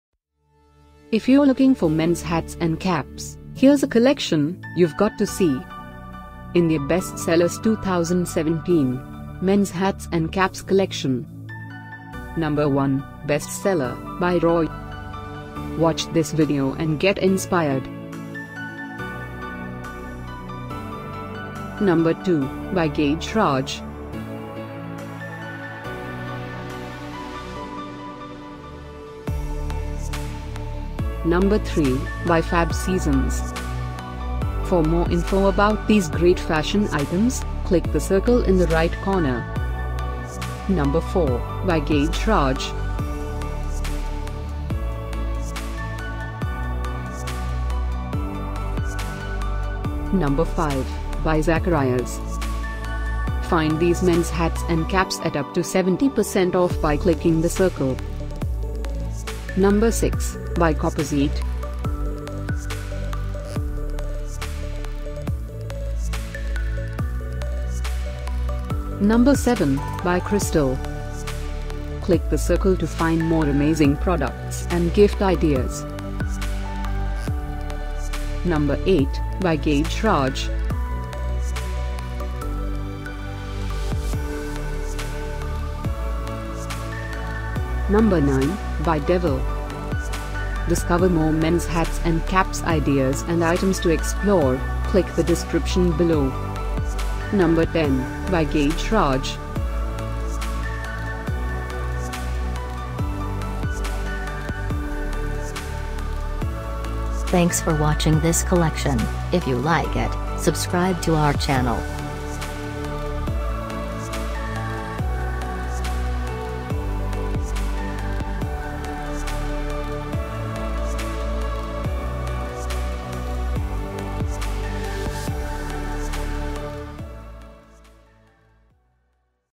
Men's Hats & Caps Collection // India Best Sellers 2017
Men's Hats & Caps Collection

Roy Unisex Cap (R_31_Black_Free Size) by Roy

Gajraj Unisex Beanie Hat (MAJ1CA_Grey_Free Size) by Gajraj

FabSeasons Cotton Skull Cap by FabSeasons

Gajraj Unisex Cotton Steel Blue Slouchy Beanie by Gajraj

Zacharias Men's Slouchy Beanie Combo of Black and Grey by Zacharias

Copperzeit Trendy Denim cap for men / women by Copperzeit

Krystle Adjustable Mesh Baseball Cap Trucker Hat Half net Plain Curved Visor Hat Black by Krystle